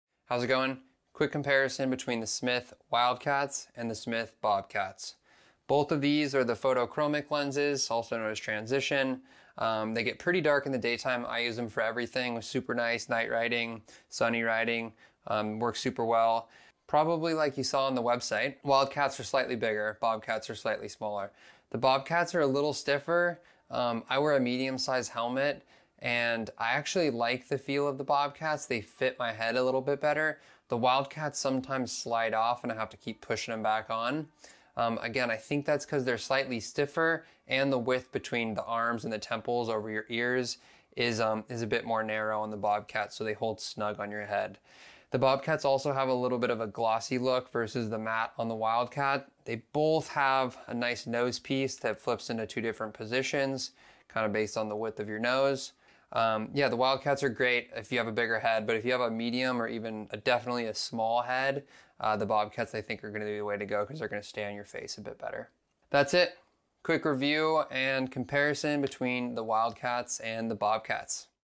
Smith Wildcat vs. Bobcat Sunglasses
I was a bit curious when purchasing the two, so I made a quick video to help others compare. Very similar in most ways but definitely quite a different feel on the face. Bobcats feel solid/snug and Wildcats feel large/relaxed for my medium helmet size head. I also found it strange that the plastic itself feels different. The Bobcats definitely are stiffer and more traditional while the Wildcats feel wayyy flexible. Hope this helps!